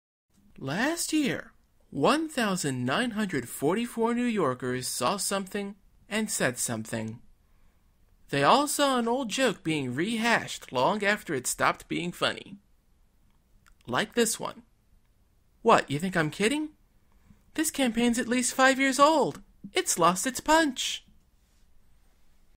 "See Something, Say Something" Spoof 5: Old Joke
A spoof of the 2007 "See Something, Say Something" TV commercials produced for the New York Metropolitan Transportation Authority. This one is in regard to old jokes.